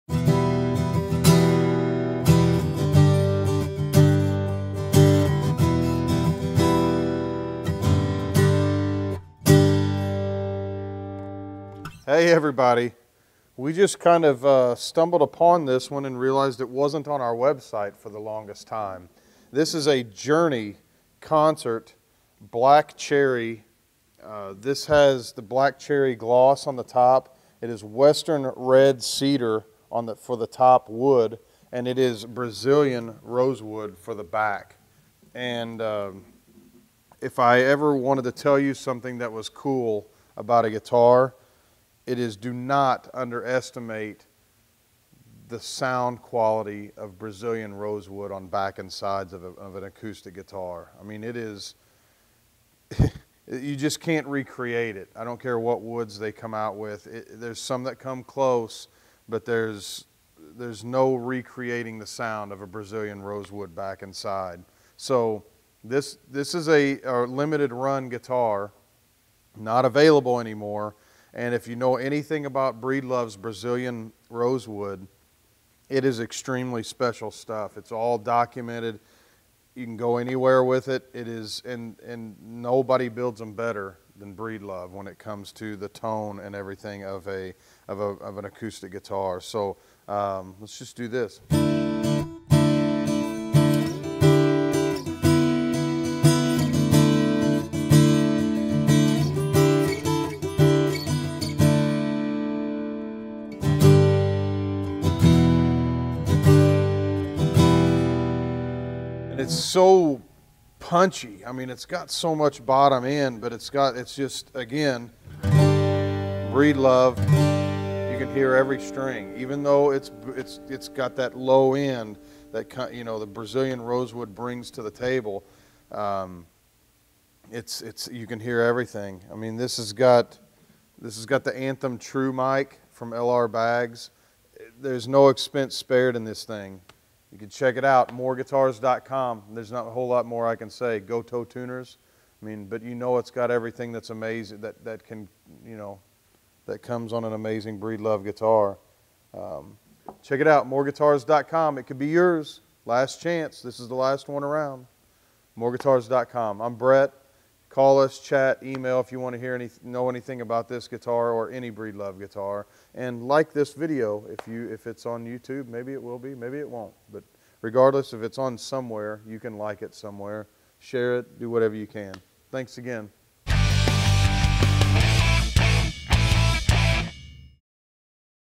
Breedlove Journey Concert FS Black Cherry FS
This Breedlove Journey Concert FS Black Cherry FS 1617 is a concert body style with a Honduran Mahogany neck. It has a solid western red cedar top with Brazilian Rosewood back and sides. The fretboard is African ebony. This Guitar is a catch. Don't miss out.